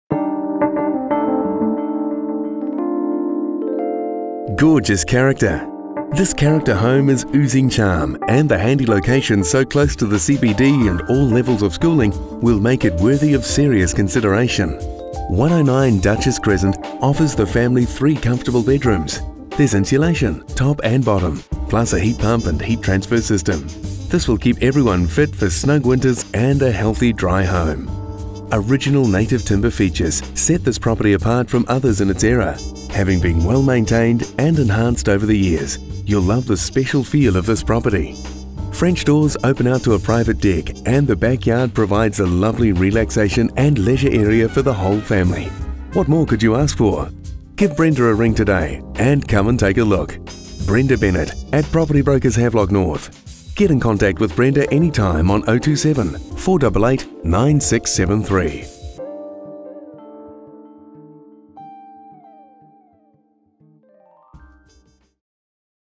109 Duchess Crescent, Mayfair.
Gorgeous Character

This character home is oozing charm, and the handy location so close to the CBD and all levels of schooling, make it worthy of serious consideration.

109 Duchess Cres, offers the family three comfortable bedrooms. There is insulation, top and bottom, plus a heat pump and heat transfer system. This will keep everyone fit for snug winters, and a healthy dry home.

Original, native timber, features set this property apart from others in its era, having been well maintained, and enhanced over the years. You will love the special feel of this property.

French doors open out onto a private deck, and the back yard provides a lovely relaxation, and leisure, area for all the family. What more could you ask for?

Call Brenda today, and come take a look!

Brenda Bennett at  Property Brokers, Havelock North.  Get in contact with Brenda anytime on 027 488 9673.